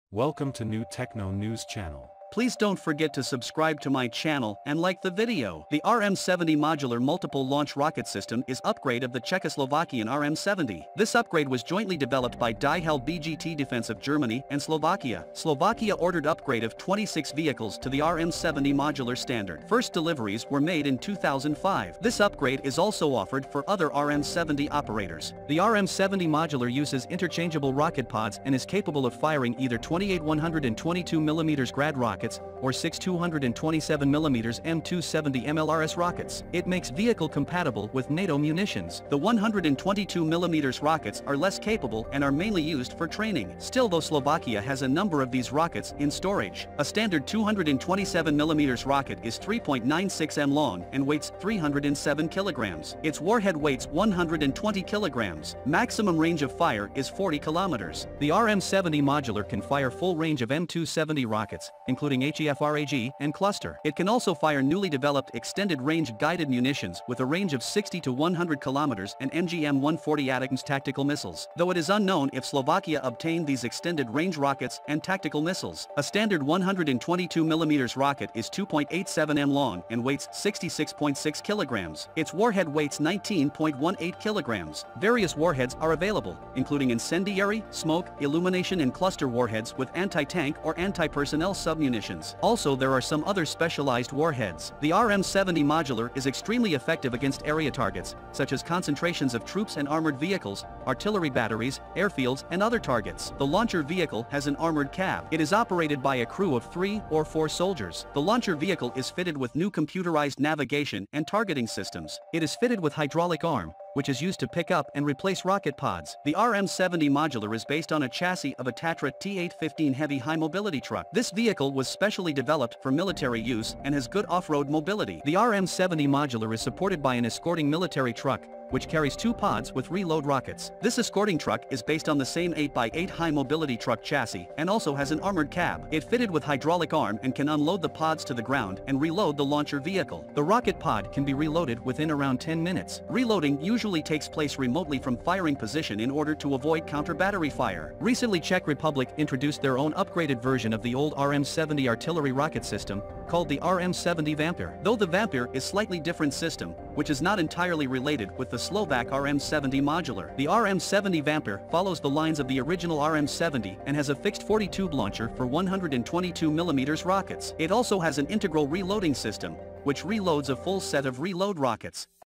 Discover RM-70 MLRS: Ukrainian Army Has Begun To Use This Rocket Launcher
RM-70 Multiple Launch Rocket System Are Already In The Service of The Armed Forces of Ukraine